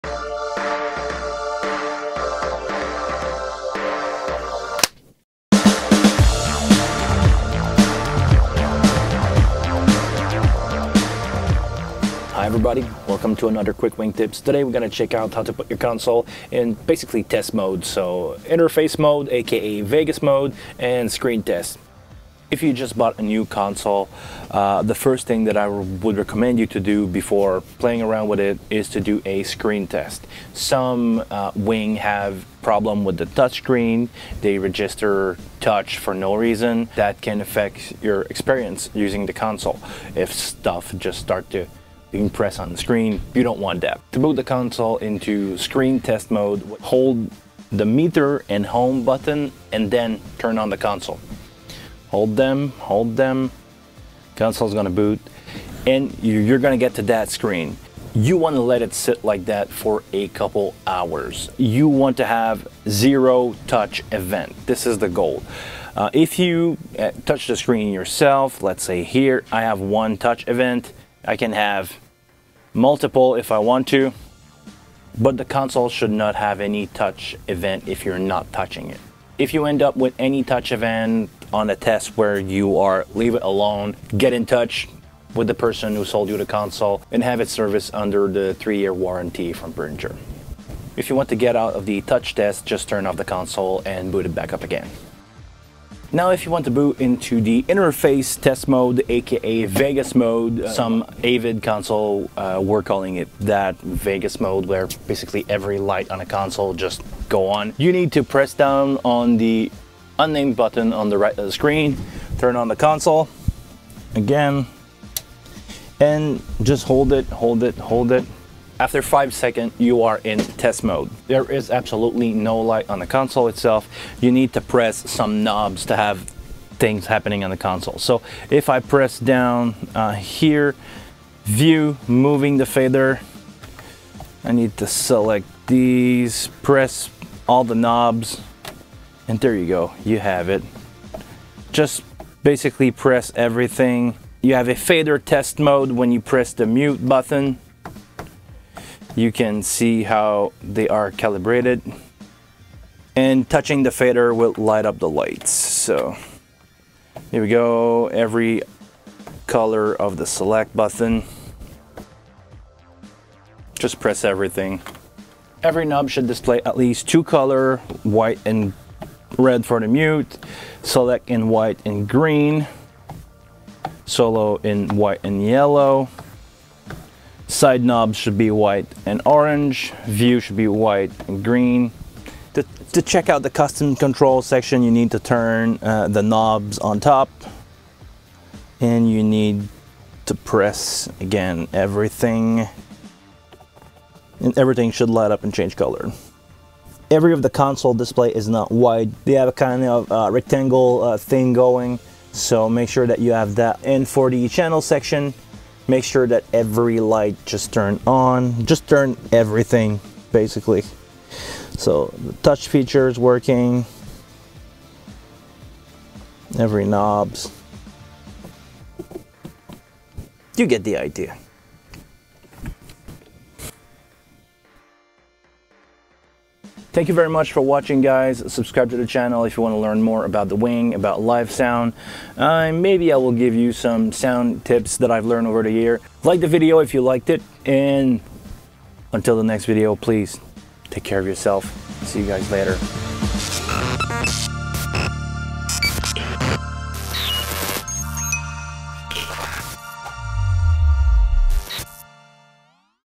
Behringer WING Hardware Test Mode - Check your Touch Screen, Faders, Knobs & LEDS
0:00 Intro
0:43 How to Screen Test
1:35 Vegas Mode / User Interface test
3:45 Watch this !